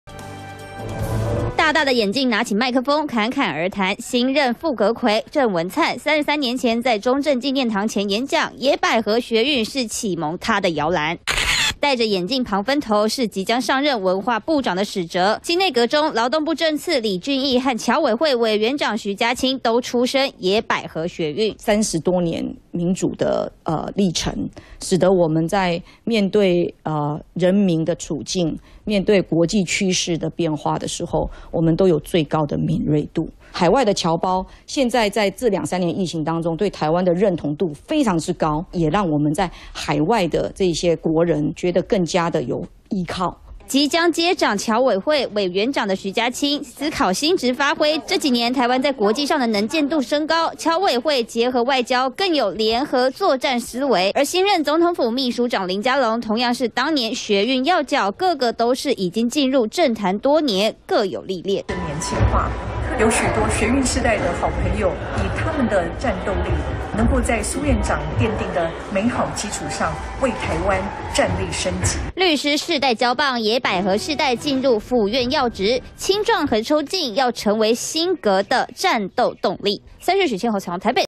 中生代接班！陳建仁內閣「野百合世代」入列│政常發揮
新任行政院長陳建仁內閣名單抵定，人事上有不少亮點！包含多位野百合學運世代的要角都入列，像是副閣揆鄭文燦、文化部長史哲和僑委會委員長徐佳青都是來自學運世代，頗有中生代接班的意味。

友邦連結區
◎ 訂閱【三立新聞網】最新影片搶先看